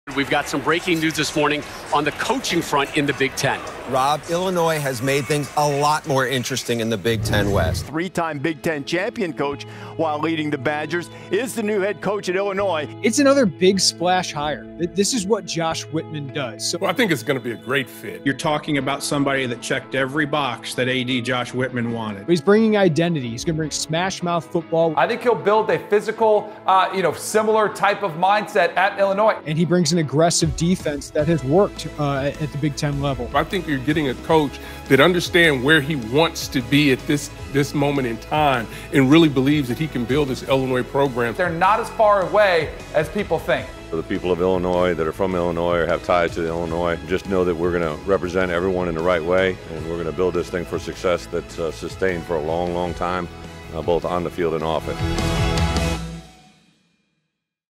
Illini Football | Media Reaction to Bielema Hire
The media reacts to Illinois' hiring of Bret Bielema as head coach of the Fighting Illini football program.